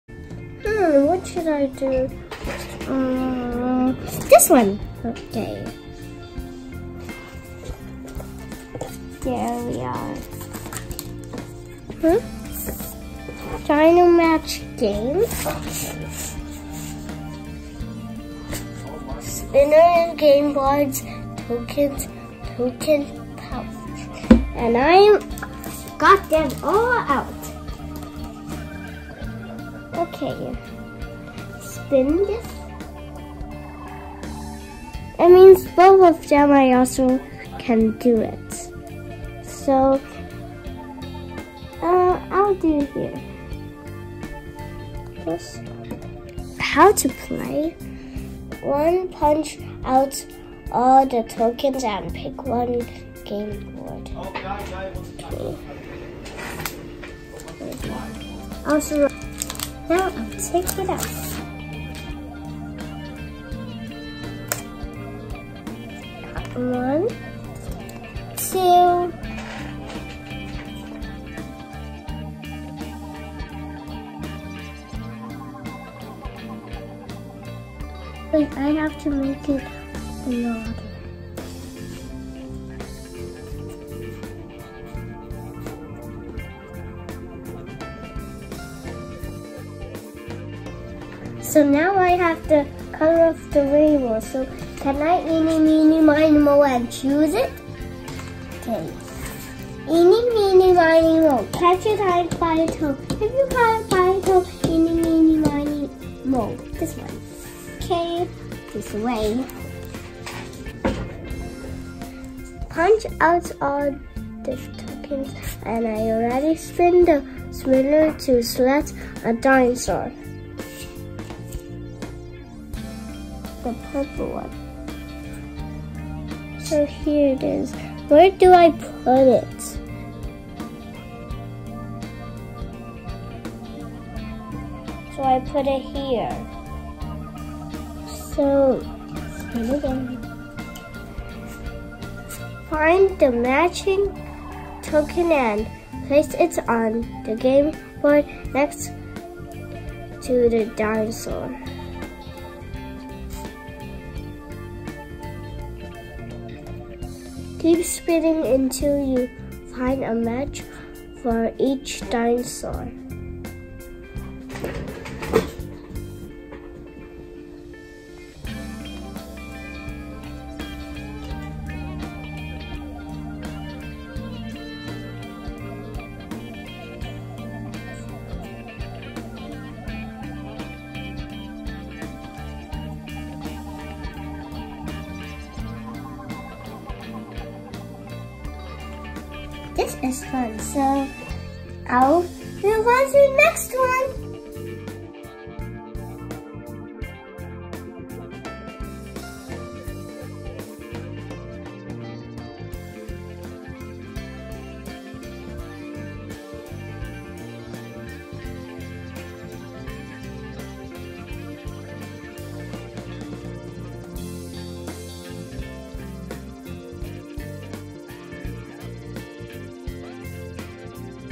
Isabella STEAM CHANNEL (6) - KIWICO DINO MATCH GAME
This video is about Isabella STEAM CHANNEL (6)  - KIWICO DINO MATCH GAME

Inspired by Ryan of "Ryan's Toys Review", Belle is trying to introduce her STEAM toys. Enjoy!